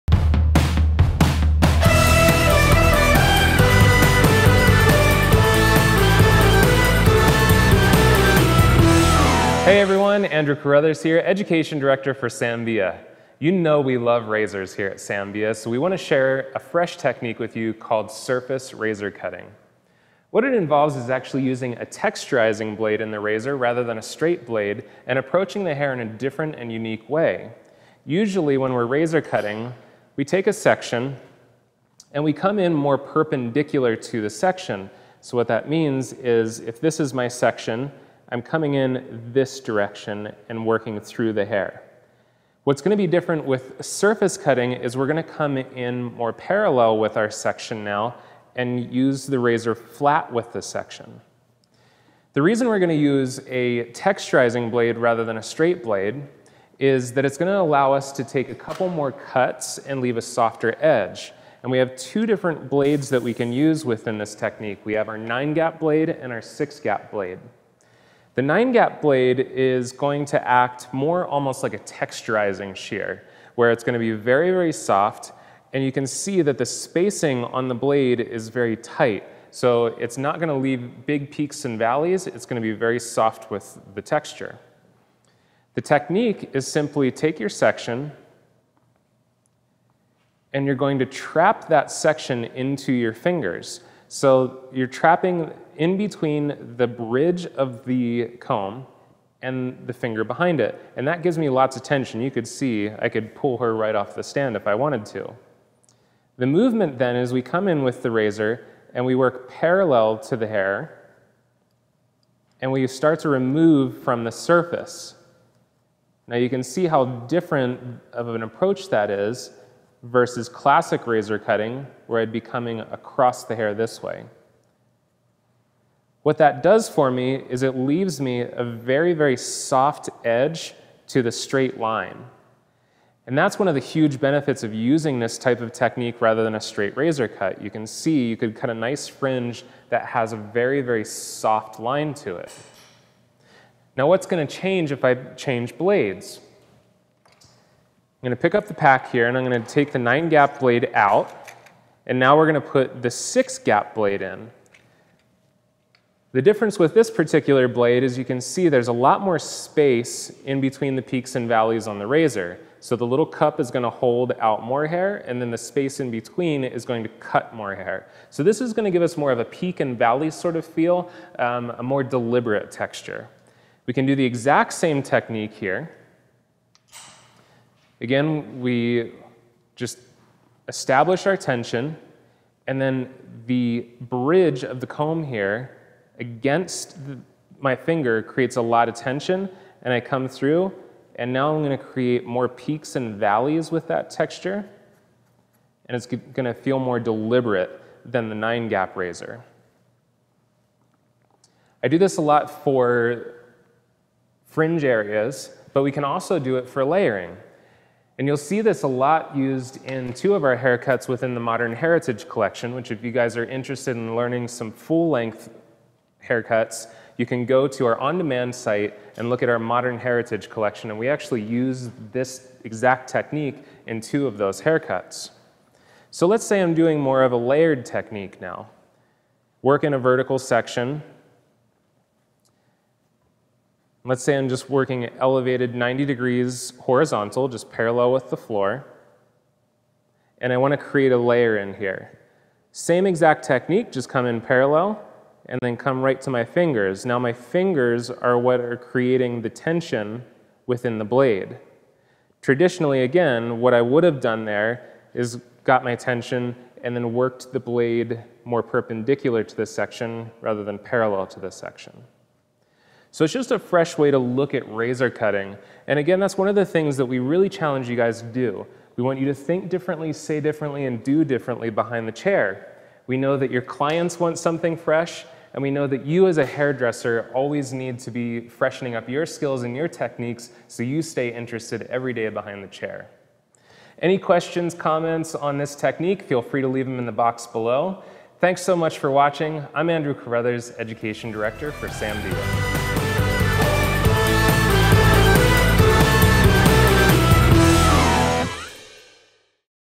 Surface Razor Cutting For a Highly Textured Finish
Stay right here, you are about to be challenged! We’re here to share an alternative and fresh approach to razor cutting on the surface of the hair. The technique of surface cutting directly relates to utilizing the razor in a parallel position to the section as opposed to a perpendicular position. All of us here at Sam Villa® just love utilizing razors in our cuts. This technique will result in straight cutting lines with a highly textured finish.

RECOMMENDED PROFESSIONAL TOOLS FOR THIS TECHNIQUE